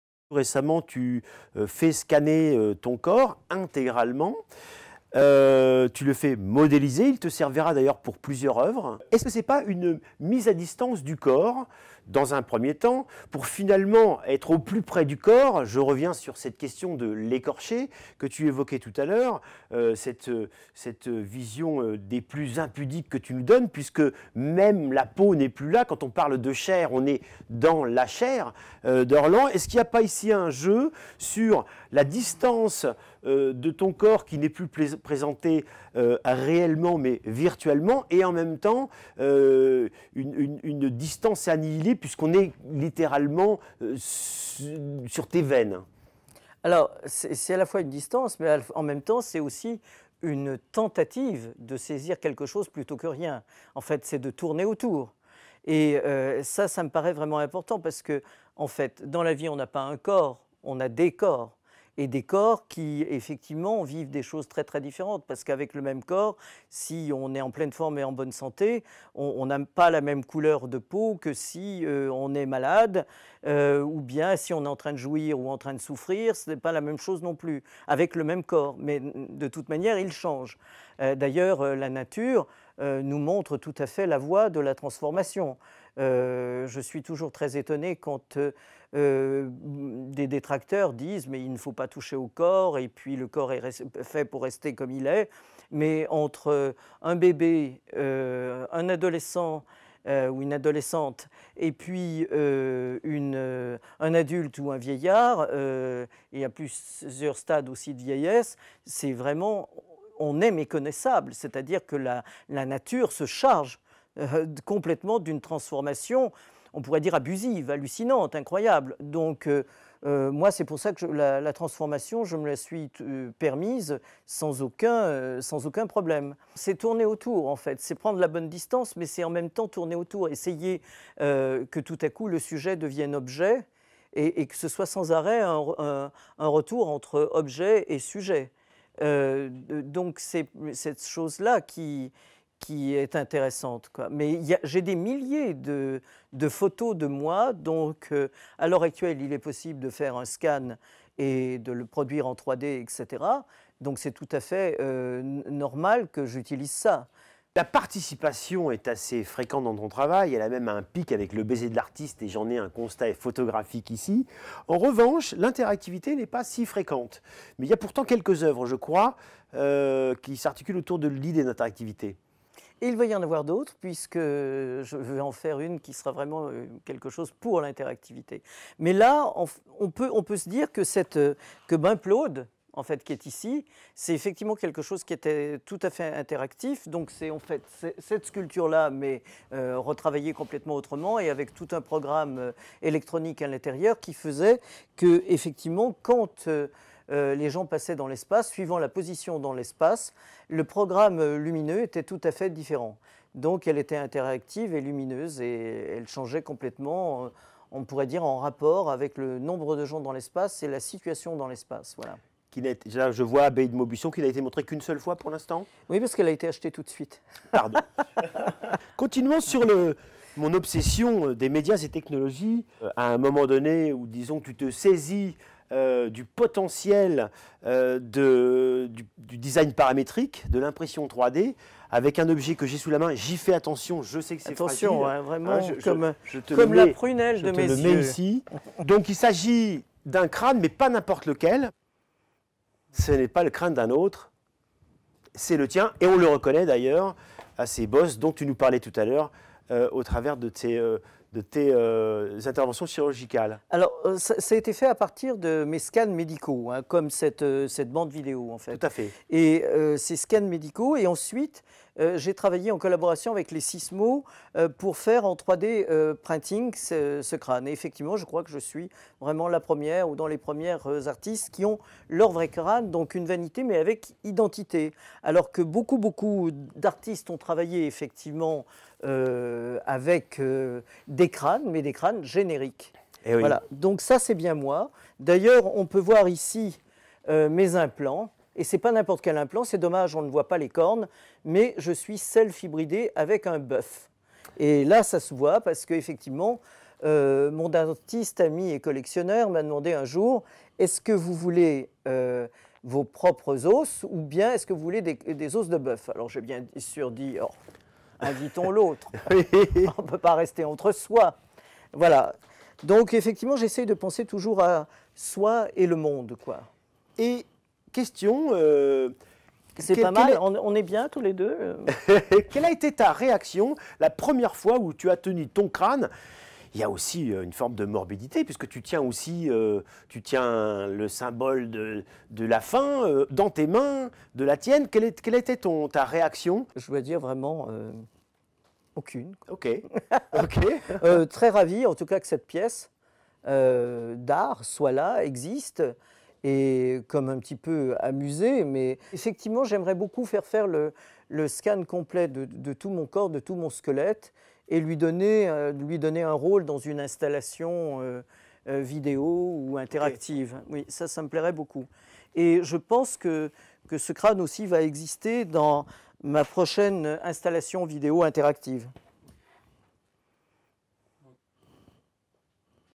ORLAN - 5. Modélisation 3D et interactivité
Scanner son corps, puis le modéliser avec une imprimante 3D, telles sont les deux manipulations entreprises par l’artiste pour mettre en lumière le sujet en tant qu’objet d’étude.
Un cours réalisé pour le MOOC Digital Media de l’École professionnelle supérieure d'arts graphiques de la Ville de Paris (EPSAA) animé par Dominique Moulon dans l’atelier d’ORLAN et coproduit par MCD.